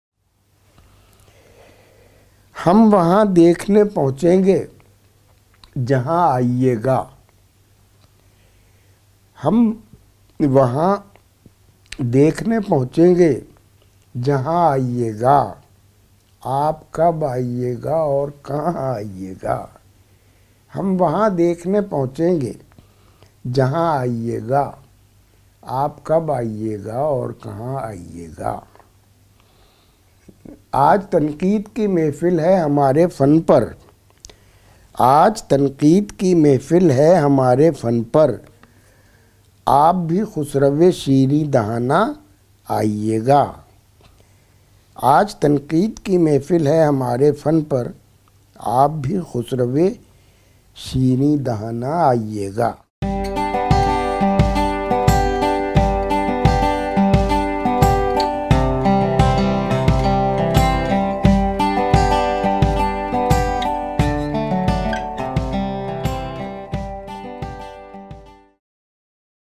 Hum waha dekhne pahochenge jaha aiyega_Ghazal by Anwar Shuoor
Anwar Shuoor pakistani poet who writes a four-lined on current affairs daily for the Pakistani Newspaper Jung. Anwar Shuoor is reciting his ghazals for Rekhta.org.